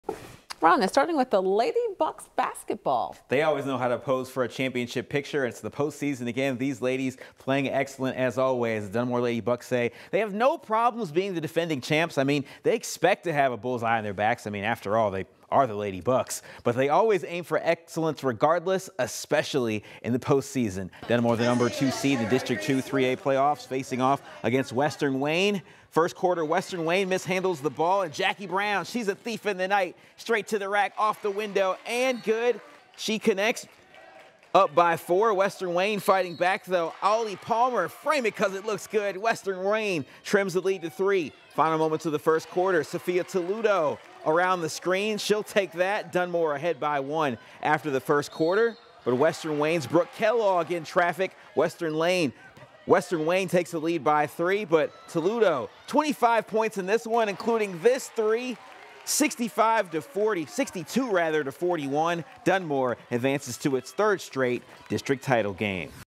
Dunmore Lady Bucks Top Western Wayne 62-41
Dunmore Advances to Third Straight District title Game